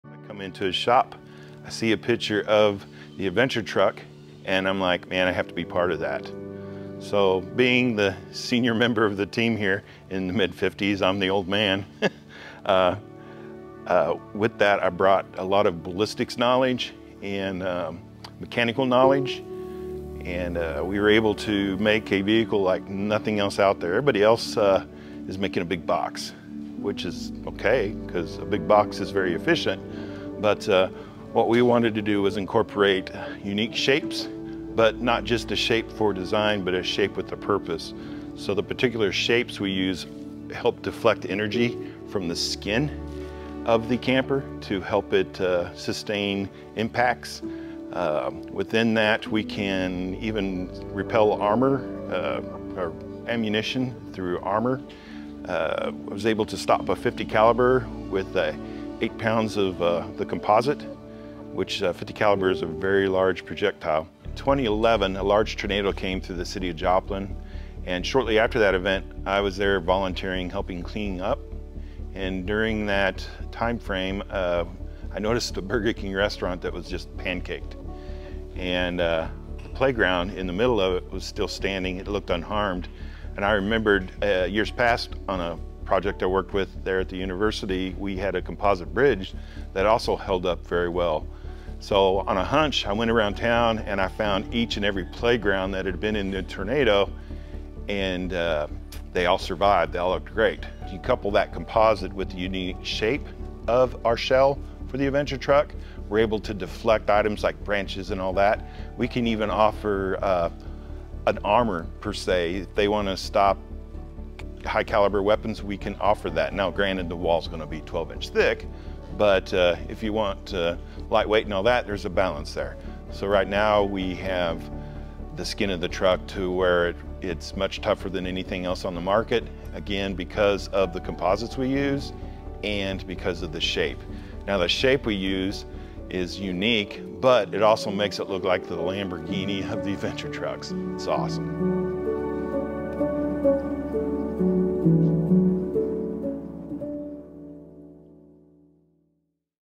Interview with Russel Gehrke 30A Ascender Expedition Truck Engineer
Behind the scenes interview with 27North Engineer Russel Gehrke, sharing with you the process and dream behind the 30A Ascender Expedition Truck.